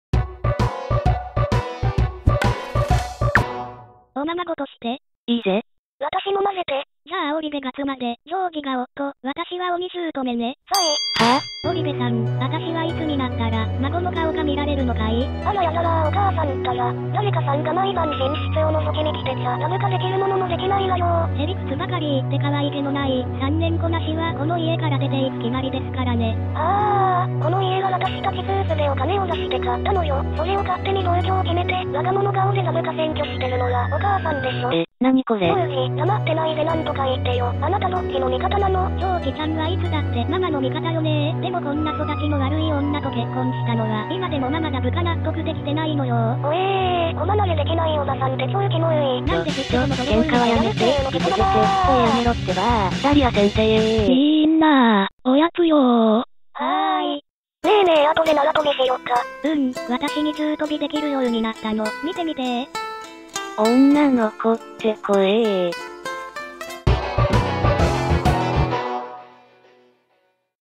キッズרוּーム #2「おままごと」 (Kizרוּm #02 "Play family") (with English CC)
皆さんどうもこんにちは、グロイ･アンダーソンと申します。
こちらはオリジナルドラマ「KIDS ⅃OOM」のおまけです。
原作タイトルはRが鏡反転して⅃(L)になる演出ですが、こちらもだぶか
タイトルの「רוּ」は「る」と読む文字なのでルが反転しても「キッズルーム」なのです。

It's nice to see you again, guys. This is Groy Anderson.
This is the extra video of the "KIDS ⅃OOM" series.
Hope you davka enjoy it!